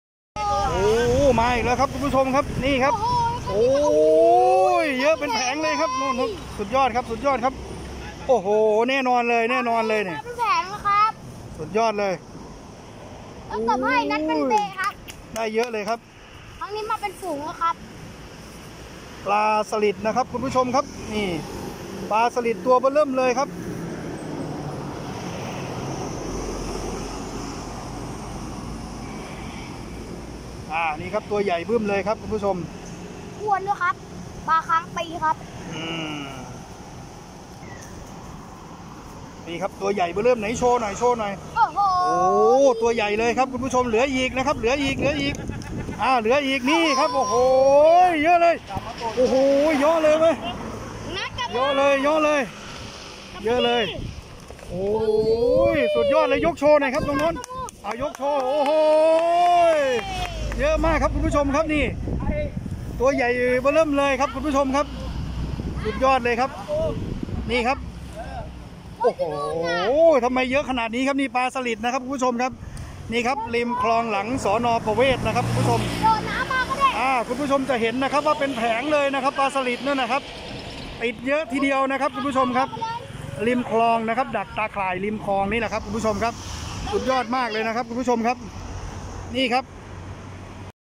โคตรปลาสลิดติดตาข่าย ใหญ่ ยักษ์มาก ลงข่ายดักปลาสลิด ใส่มองดักปลาหลัง สน.ประเวศ วิธีใช้ตาข่ายดักปลา
ใส่มอง ลงข่ายดักปลาหลัง สน.ประเวศ โคตรปลาสลิดติดตาข่าย ใหญ่ ยักษ์มาก